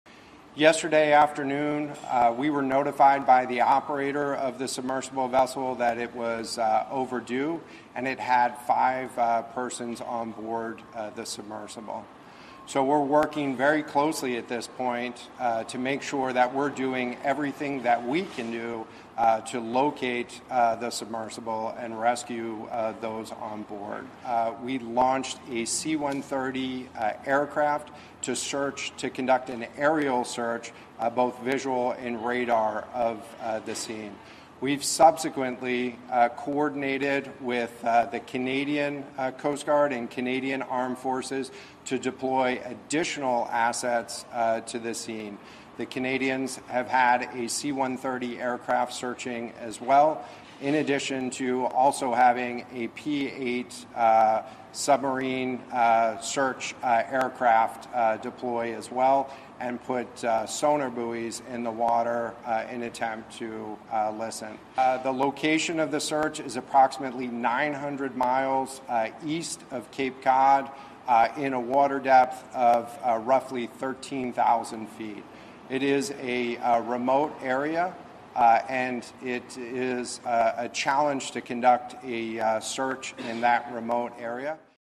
Deep-sea craft carrying 5 people to Titanic wreckage reported missing, search underway
A rescue operation by the United States Coast Guard is underway deep in the waters of the Atlantic Ocean on Monday in search of a submersible vessel that carries people to view the wreckage of the Titanic.